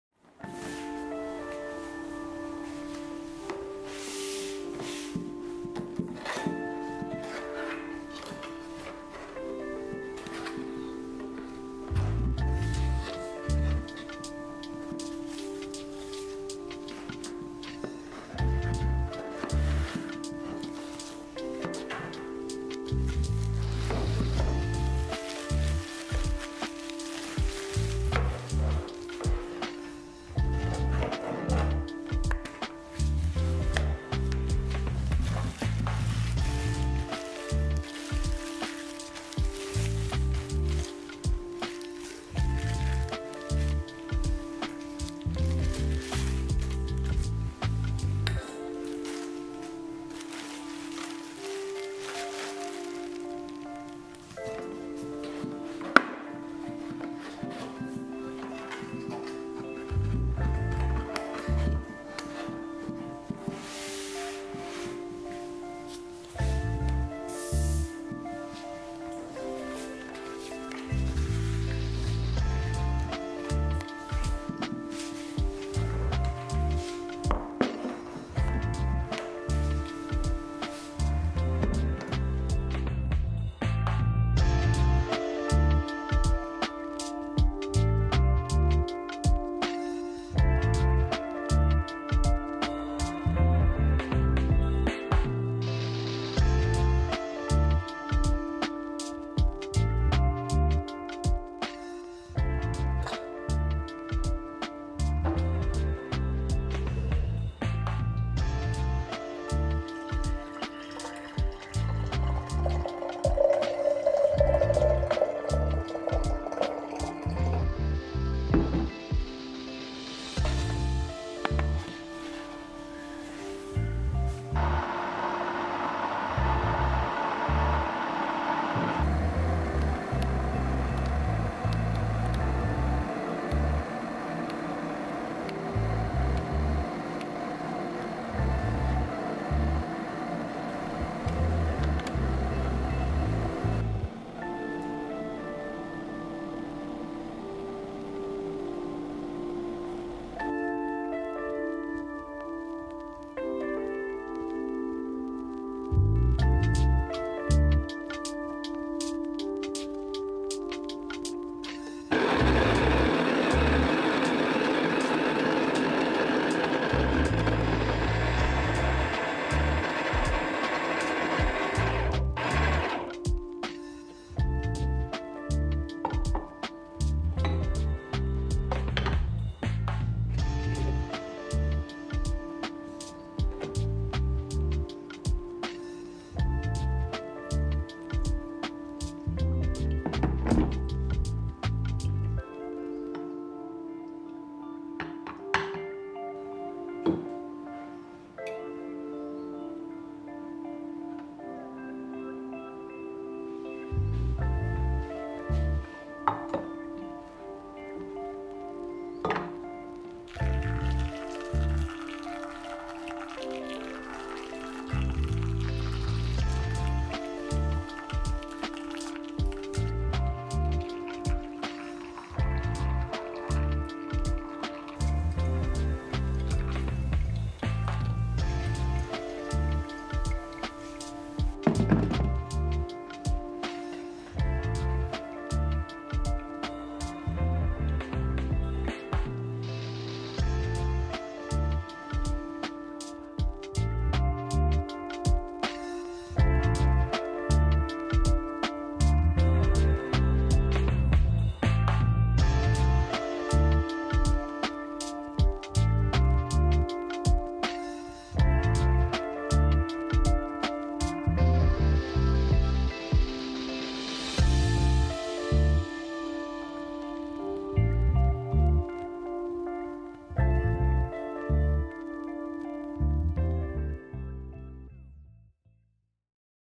[Unboxing vlog] Fellow Stagg EKG Kettle + First try
The EKG is simple and intuitive to use because we also can quickly achieve water heated to our exact desired temperature.. amazing!
The design is so sleek and stylish.. so worth of money :)

(First try with Origami Dripper, Origami Dripper Wooden Holder, Grinder Wilfa, Torch Coffee Server Pitchii, THF coffee beans, fritz coffee company's cupping cup)